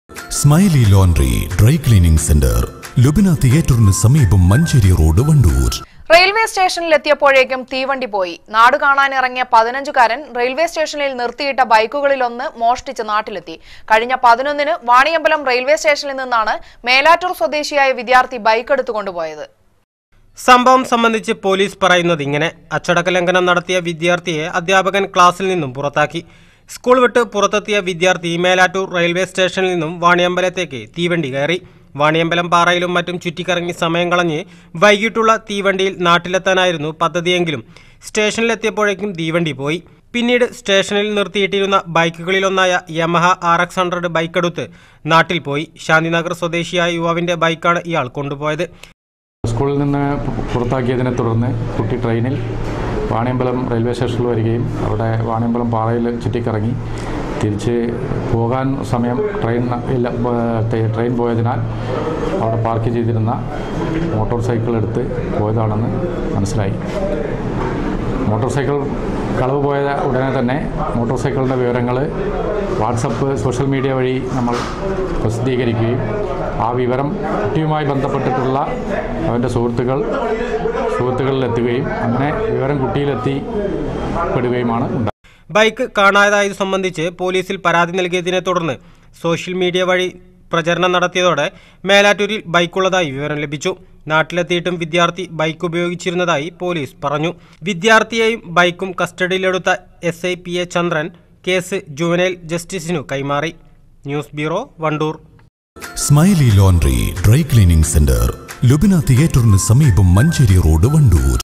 sahya news
റെയില്‍വേ സ്റ്റേഷനിലെത്തിയപ്പോഴേക്കും തീവണ്ടി പോയി.
നാടു കാണാനിറങ്ങിയ പതിനഞ്ചുകാരന്‍ തിരിച്ച് നാട്ടിലെത്തിയത് റെയില്‍വേ സ്റ്റേഷനില്‍ നിന്ന് ബൈക്ക് മോഷ്ടിച്ച്.
വാണിയമ്പലം റെയില്‍വേ സ്റ്റേഷനില്‍ നിന്നാണ് മേലാറ്റൂരിലെ വിദ്യാര്‍ഥി ബൈക്കെടുത്തത.്